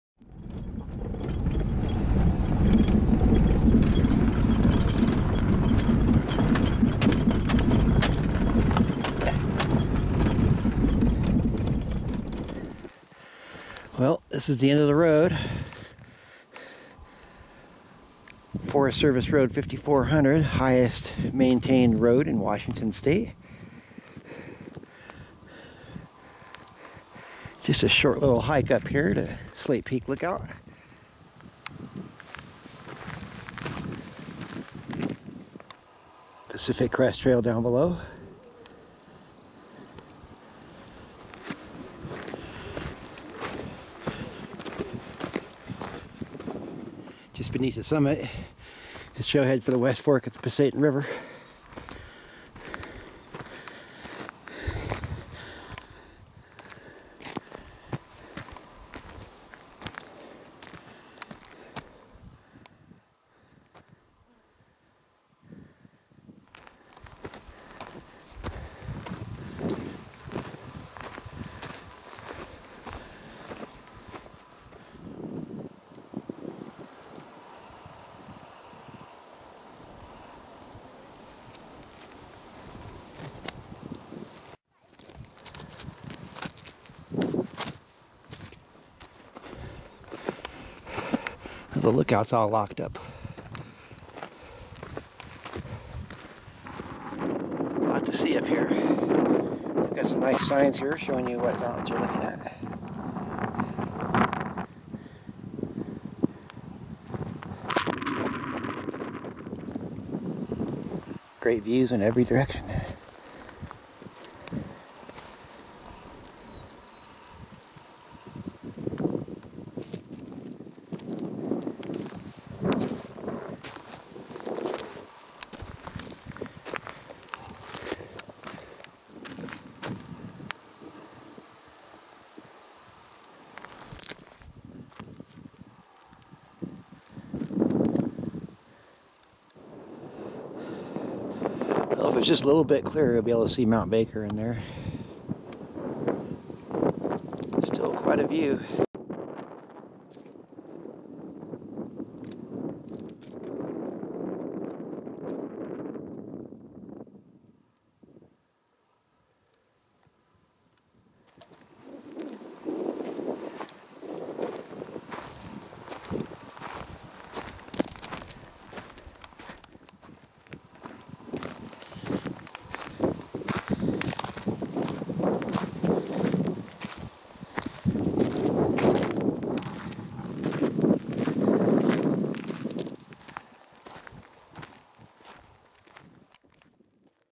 Slate Peak
July 14, 2016. Highest maintained road in Washington State.

The peak was a fire lookout, then during the Cold War the military blasted 40 feet off the top to make it into a radar station. Then they changed their minds and gave it back to the Forest Service. The lookout was rebuilt on a 40-foot tower to give it the same view as it had before. Now the lookout is locked up, as it's not structurally safe.